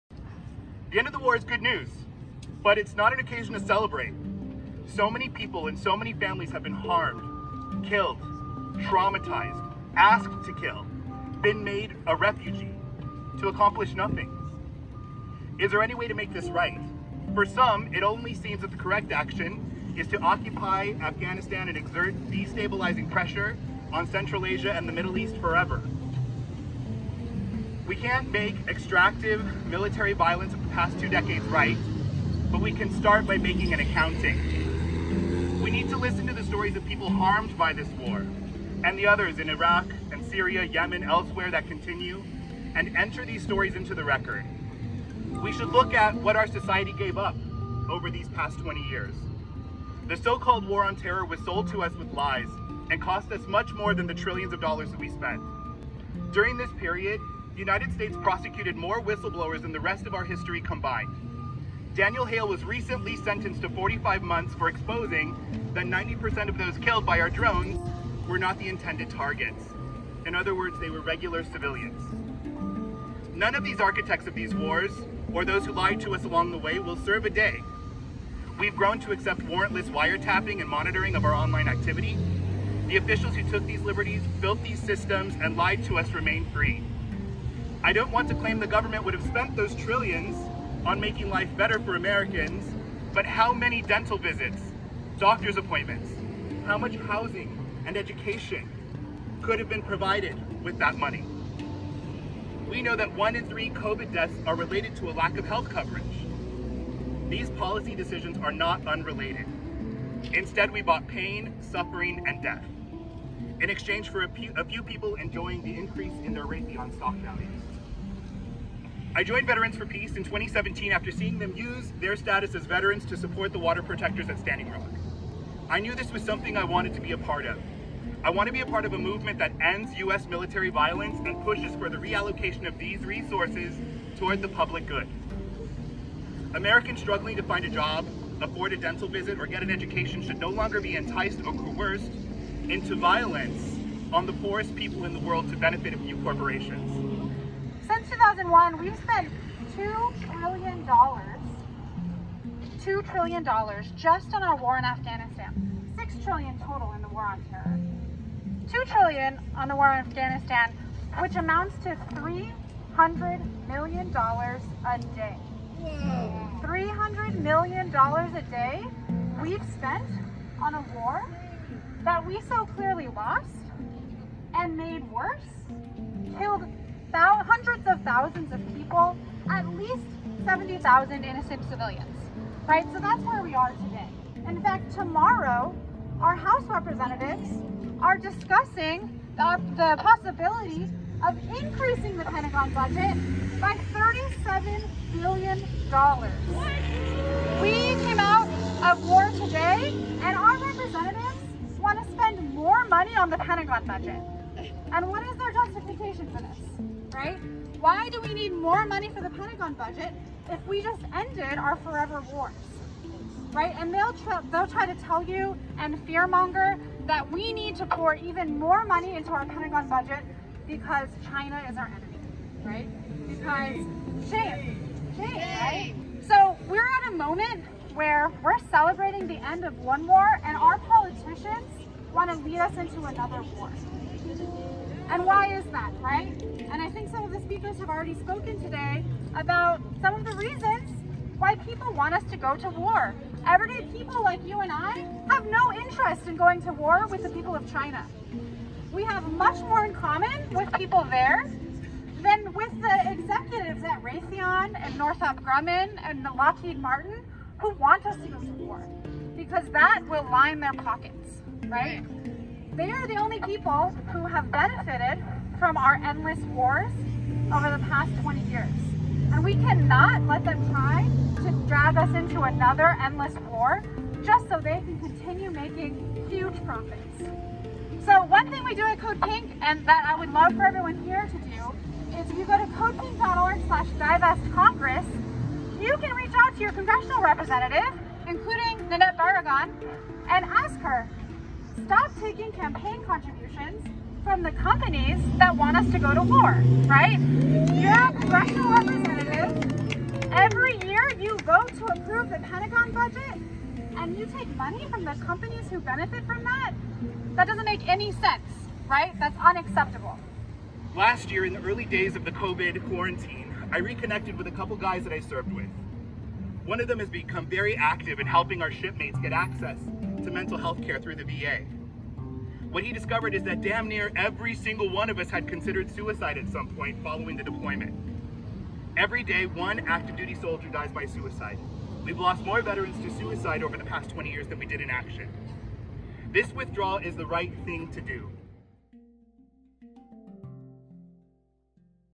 LA Harbor Peace Week: The End of a 20-year War
LA Harbor Peace Week kicked off with CODEPINK and Veterans for Peace, two antiwar organizations demanding reparations for Afghan people and an end to militarism.

The rally took place on Aug. 31, near the corner of Centre and 5th Streets. The day marked the deadline for U.S. forces to pull out of Afghanistan, ending a 20-year war with the Taliban.

“We are here celebrating the end of the war and saying that war was never the answer in the first place,” said Rachel Bruhnke, director of LA Harbor Peace Week. “It's not over for the Afghan people, and it's not over for the traumatized veterans.”